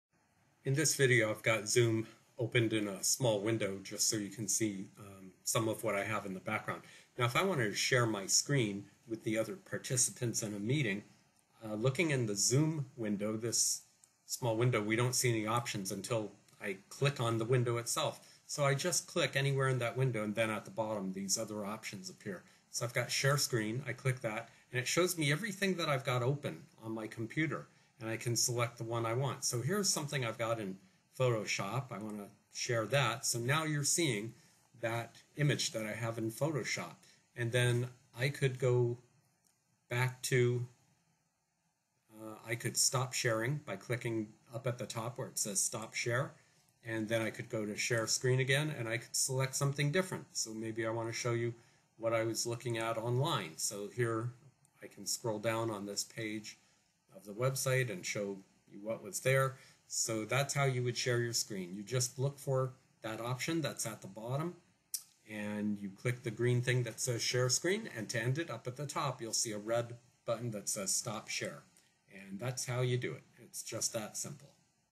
How to Share Your Screen in Zoom
Our business referral group uses Zoom to conduct our networking events. We meet each week to pass referrals to each other.